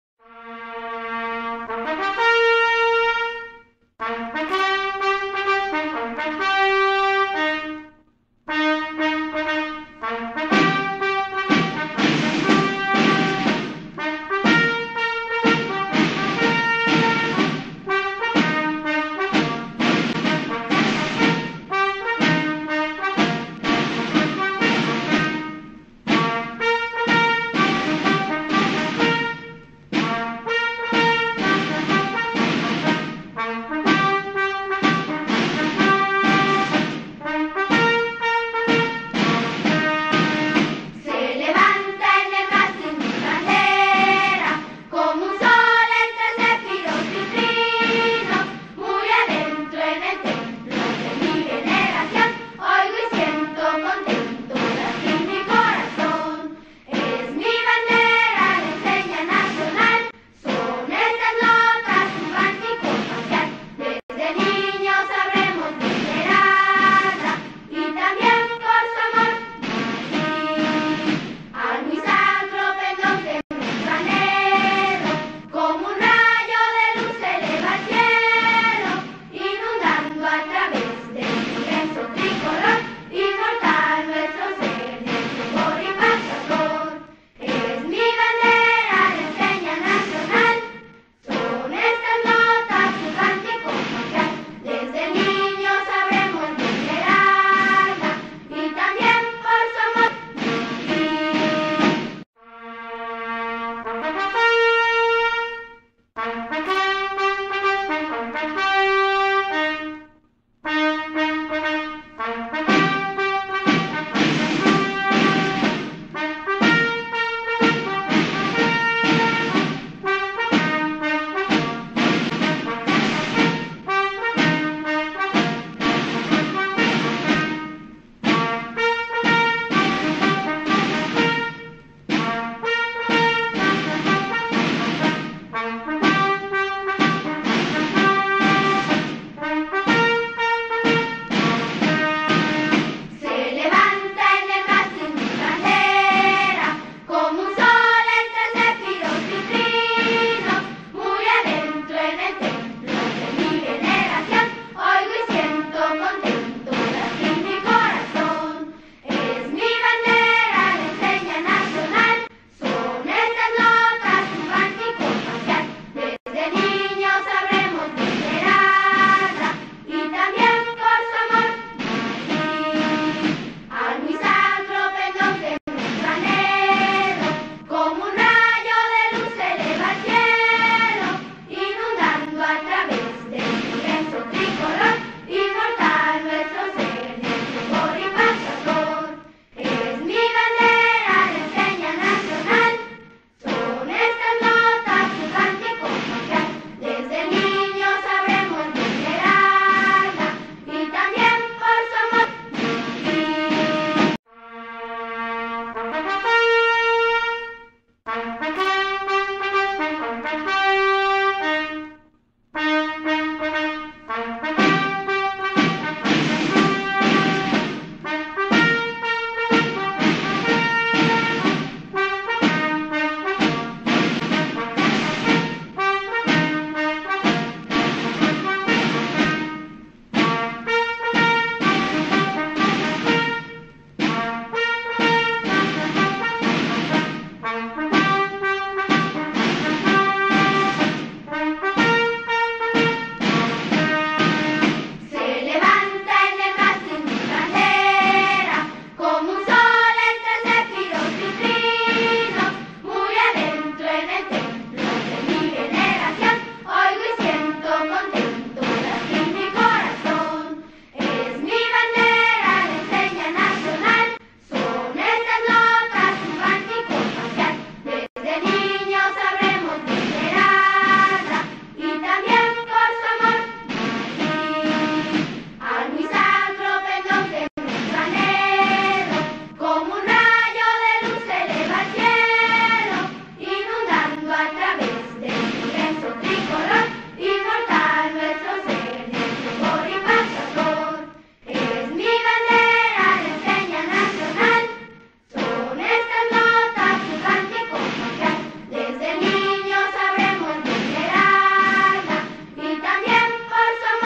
FESTEJO DIA DE LA BANDERA EDICION 2019 PABELLON
DOMINGO 24 DE FEBRERO DEL 2019 DIA DE NUESTRO LABARO PATRIO FESTEJO HERMOS REALIZADO POR LA SECUNDARIA TECNICA 23 DE PABELLON DE ARTEAGA, AGS. UN GRAN EVENTO